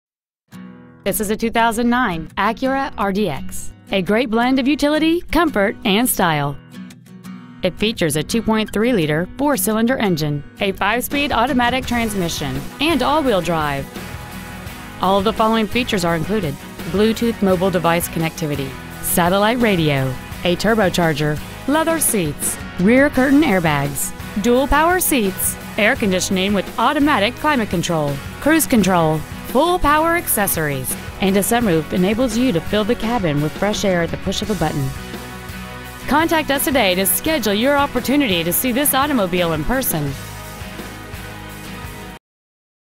2009 Acura RDX Lafayette LA
Year : 2009
 Make : Acura
 Model : RDX
 Engine : 2.3L 4 cyls
 Trans . : Automatic
 Exterior : Gray
 Miles : 101,864

 Stock : 142010A